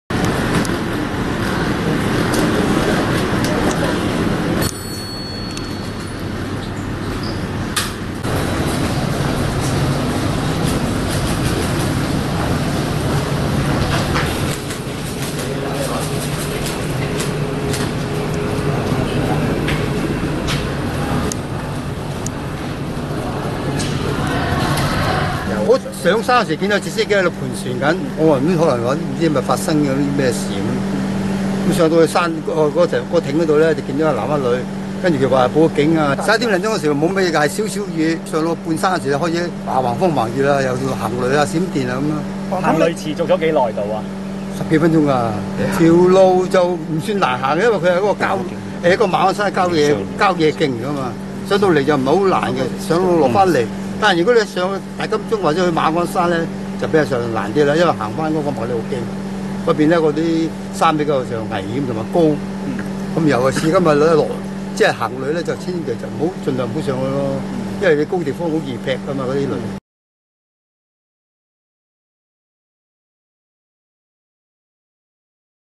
行山遇雷劈：死者將升讀大學 性格外向熱愛戶外運動
行山遇雷劈的姓盧死者，洋名Ian，就讀南島中學，該所國際學校位於香港仔南風道，為英基學校協會旗下，而盧即將升上大學，豈料遭遇不測。英基學校協會發言人指，今次意外非常不幸，校方向Ian的家人及朋友致以最深切的慰問。在這非常艱難的時刻，校方會提供一切所需要的支援和協助。

死者的家人對事件感到十分惋惜和傷心，叔公表示，Ian是家中長子，生前「好孝順」，亦十分好動外向，熱愛運動以及戶外活動，平日打棒球、踏單車，偶爾會相約朋友行山，近日父母到日本旅遊，他們已獲悉事件，並即時買機票返港。

據當時正在行山的張先生指，今日曾看過天文台的告示，只看到會下雨，但沒有留意到有雷暴警告，故如常出發。至中午12時許，天氣突然轉壞，形容：「落好大雨，行雷閃電好犀利，持續咗10幾20分鐘，好恐怖！原本我都冇停照行，但見勢色唔對，唯有落番嚟。」他見此即時到附近的一個涼亭暫避。

期間見到一男一女焦急地打電話，他上前查問，他們表示其友人受了傷，正等待救援；但當時並不知道傷者是遭雷劈。張稱，據自己多年的行山經驗，若遇上行雷閃電，不應繼續行，應找個「有瓦遮頭」避吓，以免發生意外。